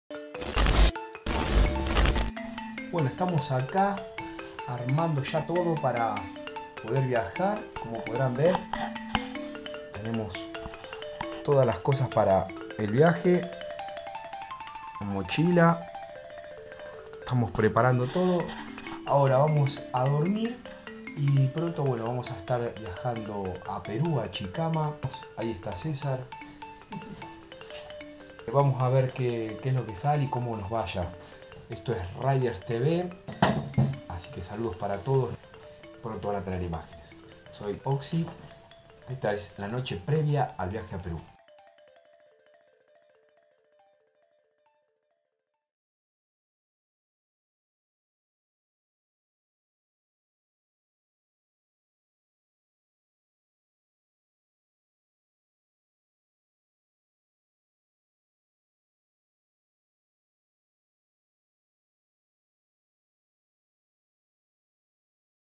Noche previa parte 1 Viaje A Chicama, Perú
En esta ocacion les traigo mi aventura por Chicama donde esta la ola más larga del mundo en Perú espero lo disfruten!! espero lo disfruten nos vemos !